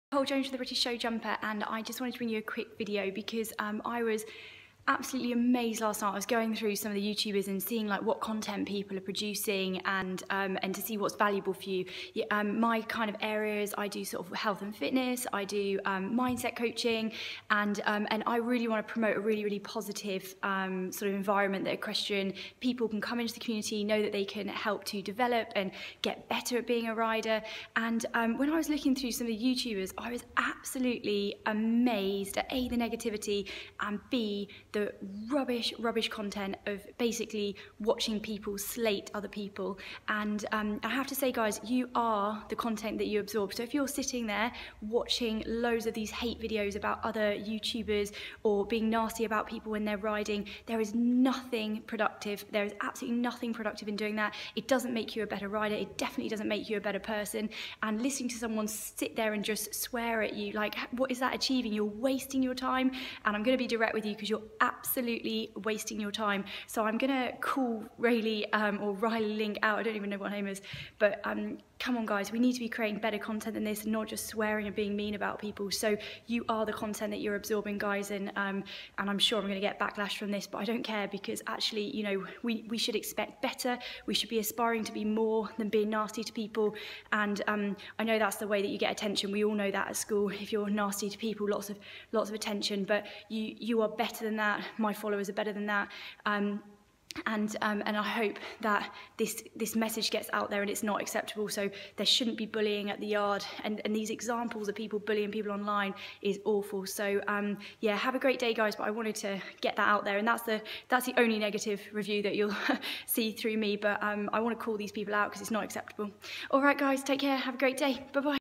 RayleighLink14 Stop absorbing content that promotes bullying and hate.
You are the content you absorb.

Do you really want to waste your time absorbing that is negative and abusive?